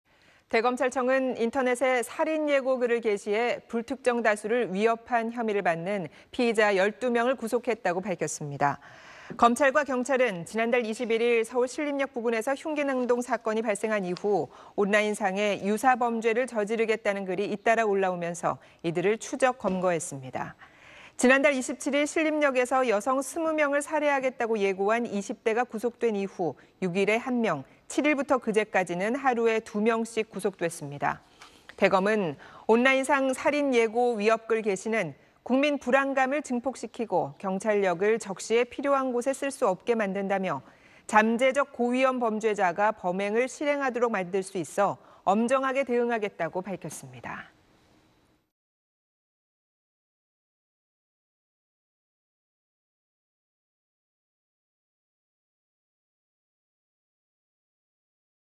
전국서 '살인 예고' 12명 구속…절반 이상 20대 이하 / SBS
대검찰청은 인터넷에 '살인 예고' 글을 게시해 불특정 다수를 위협한 혐의를 받는 피의자 12명을 구속했다고 밝혔습니다.

검찰과 경찰은 지난달 21일 서울 관악구 신림역 부근에서 흉기 난동 사건이 발생한 이후 온라인상에 유사 범죄를 저지르겠다는 글이 잇따라 올라오면서 이들을 추적·검거했습니다.

지난달 27일 신림역에서 여성 20명을 살해하겠다고 예고한 20대가 구속된 이후 6일에 1명, 7일부터 그제까지는 하루에 2명씩 구속됐습니다.

대검은 "온라인상 살인 예고 위협글 게시는 국민 불안감을 증폭시키고 경찰력을 적시에 필요한 곳에 쓸 수 없게 만든다"며 "잠재적 고위험 범죄자가 범행을 실행토록 만들 수 있어 엄정하게 대응하겠다"고 밝혔습니다.